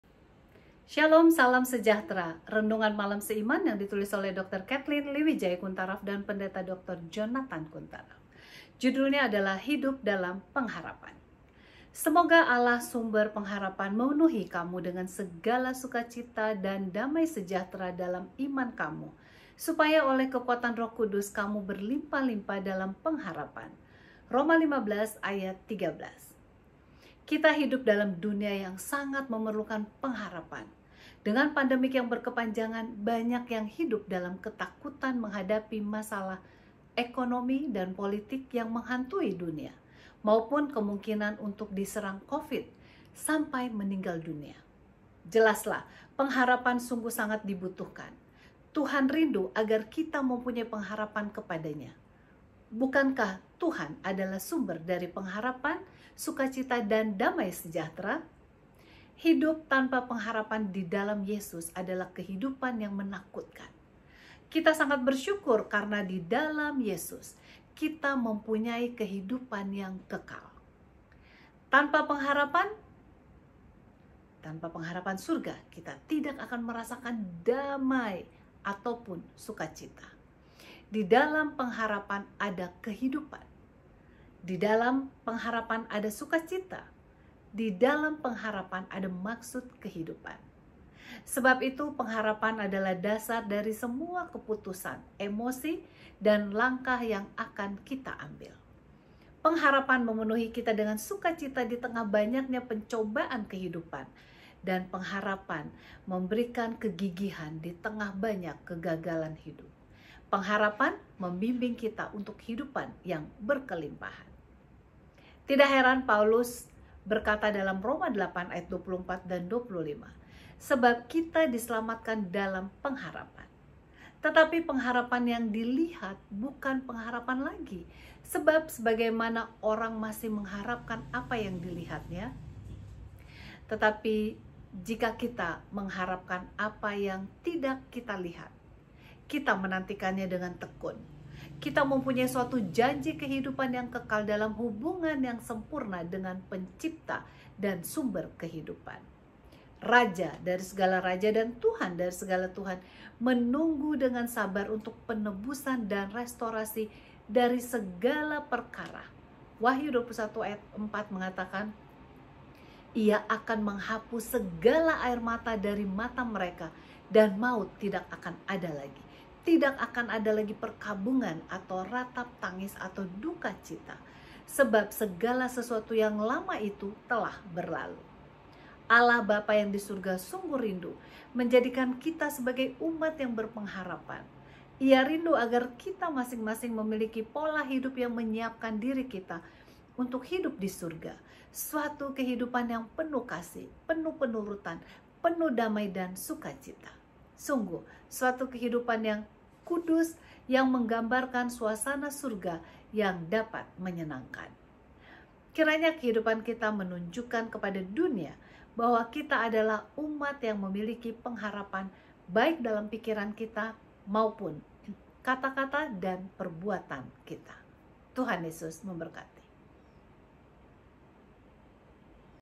#1479 - HIDUP DALAM PENGHARAPAN | Renungan Malam Seiman | Peggy Iskandar Wowor
RMS @peggyiskandarwowor
Video: Peggy Iskandar Wowor
Penulis:
Dr. Kathleen Liwidjaja Kuntaraf
Pdt. DR. Jonathan Kuntaraf

18 Januari 2022

HIDUP DALAM PENGHARAPAN

Semoga Allah, sumber pengharapan, memenuhi kamu dengan segala sukacita dan damai sejahtera dalam iman kamu, supaya oleh kekuatan Roh Kudus kamu berlimpah-limpah dalam pengharapan. Roma 15:13.

Kita hidup dalam dunia yang sangat memerlukan  pengharapan. Dengan pandemik yang berkepanjangan, banyak yang hidup dalam ketakutan menghadapi masalah ekonomi dan politik yang menghantui dunia, maupun kemungkinan untuk diserang Covid sampai meninggal dunia. Jelaslah   pengharapan sungguh sangat dibutuhkan. Tuhan rindu agar kita mempunyai pengharapan kepadaNya. Bukankah Tuhan adalah sumber dari pengharapan, sukacita dan damai sejahatera?

Hidup tanpa pengharapan di dalam Yesus adalah kehidupan yang menakutkan. Kita sangat bersyukur sebab di dalam Yesus, kita mempunyai kehidupan yang kekal. Tanpa pengharapan surga, kita tidak akan merasakan damai, ataupun sukacita. Di dalam pengharapan ada kehidupan. Di dalam pengharapan ada sukacita. Di dalam pengharapan ada maksud kehidupan. Sebab itu pengharapan adalah dasar dari semua keputusan, emosi dan langkah yang kita ambil. Pengharapan memenuhi kita dengan sukacita di tengah banyaknya pencobaan kehidupan, dan pengharapan memberikan kegigihan di tengah banyak kegagalan hidup. Pengharapan membimbing kita untuk kehidupan yang berkelimpahan.
Tidak heran Paulus berkata dalam Roma 8:24, 25, “Sebab kita diselamatkan dalam pengharapan. Tetapi pengharapan yang dilihat, bukan pengharapan lagi; sebab bagaimana orang masih mengharapkan apa yang dilihatnya? Tetapi jika kita mengharapkan apa yang tidak kita lihat, kita menantikannya dengan tekun.”

Kita mempunyai suatu janji kehidupan yang kekal dalam hubungan yang sempurna dengan Pencipta dan Sumber Kehidupan. Raja dari segala raja dan Tuhan dari segala Tuhan menunggu dengan sabar untuk penebusan dan restorasi dari segala perkara, Wahyu 21:4 mengatakan, “Ia akan menghapus segala air mata dari mata mereka, dan maut tidak akan ada lagi; tidak akan ada lagi perkabungan, atau ratap tangis, atau dukacita, sebab segala sesuatu yang lama itu telah berlalu.”

Allah Bapa yang di surga sungguh rindu menjadikan kita sebagai umat yang berpengharapan. Ia rindu agar kita masing masing memiliki pola hidup yang menyiapkan diri kita untuk hidup di surga, suatu kehidupan yang penuh kasih, penuh penurutan, penuh damai dan sukacita. Sungguh suatu kehidupan yang kudus yang menggambarkan suasana surga yang sangat menyenangkan.

Kiranya kehidupan kita menunjukkan kepada dunia bahwa kita adalah umat yang memiliki pengharapan baik dalam pikiran kita, maupun kata-kata dan perbuatan kita.